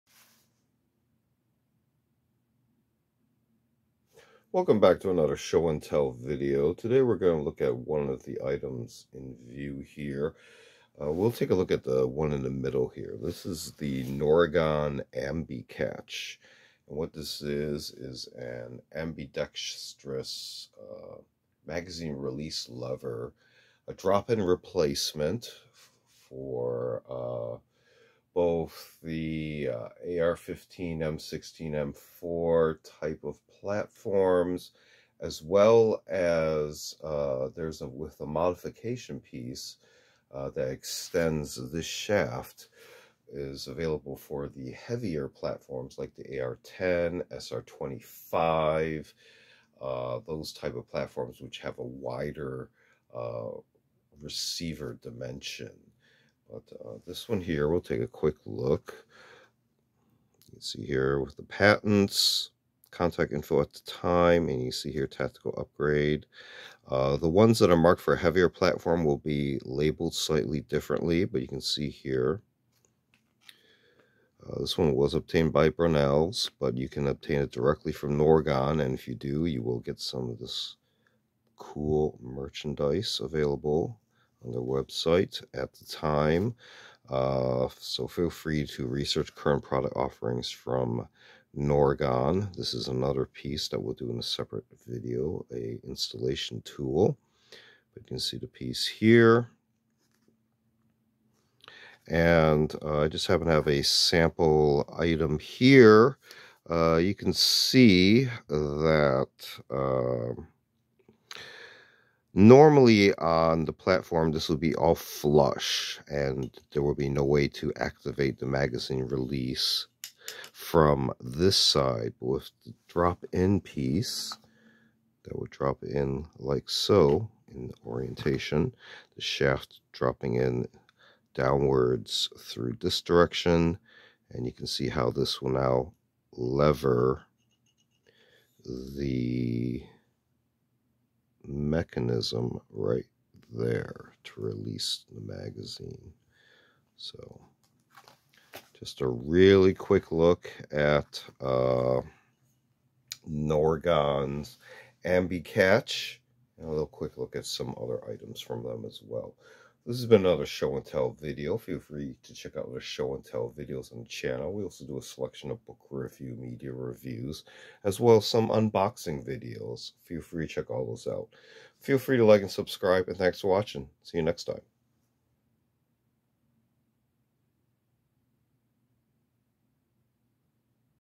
NORGON, LLC Ambi- Catch Ambidextrous Magazine Release Mechanism, M16, AR15 (M4)
SHOW AND TELL: NORGON, LLC Ambi- Catch Ambidextrous Magazine Release Mechanism
M-16, AR-15, M-4
(also not shown version for SR-25, AR-10 etc)